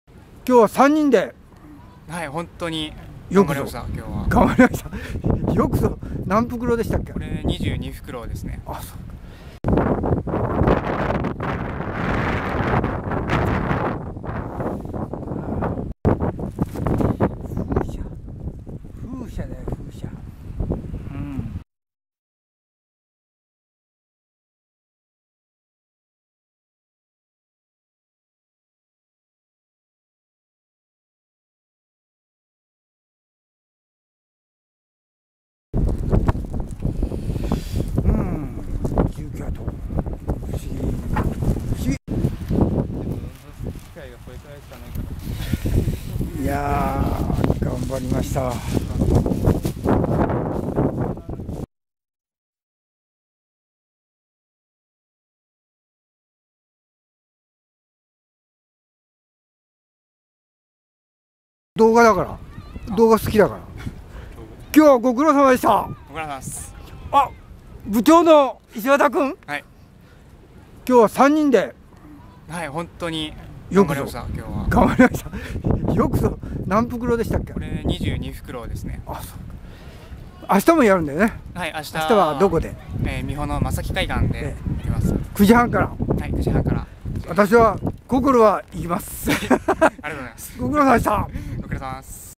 第一回E C Oｸﾘｰﾝｵｹｰｼﾞｮﾝ2015年10月17日大浜海岸
少人数でもごみがすぐ集る！不法投棄の塊だ、ペットボトルは散乱して・・・
第一回E.C.Oクリーンオケージョン　2015年10月17日
大浜海岸　ｂｙ東海大海洋学部環境サークルE.C.O
皆さん！ご苦労様＼(^o^)／